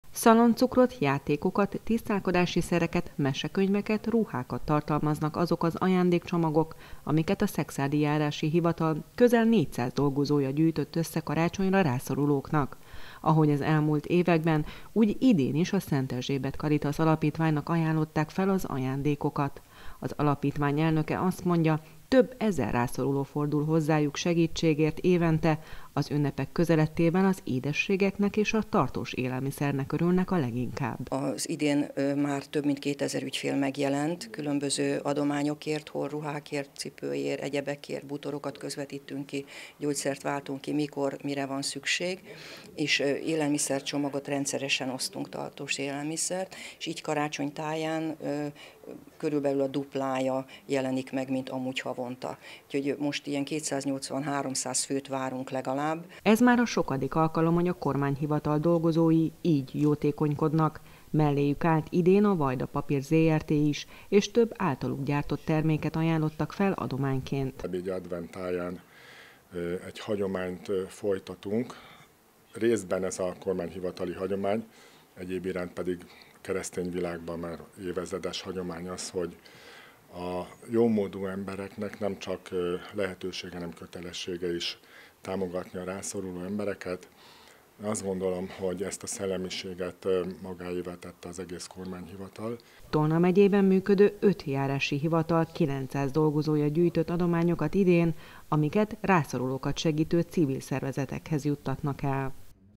Jótékonykodás - 900 közalkalmazott gyűjtötte az adományokat a karitásznak 20181213
Tolna megyében 900  közalkalmazott gyűjtött adományokat, hogy szebbé tegyék a rászoruló családok karácsonyát. A szekszárdi járási hivatal által gyűjtött ajándékcsomagokat a Szent Erzsébet Caritas Alapítványnak ajánlották fel. A szekszárdi szervezetet évente 3 ezren keresik fel, különösen év végén nő meg a hozzájuk fordulók száma.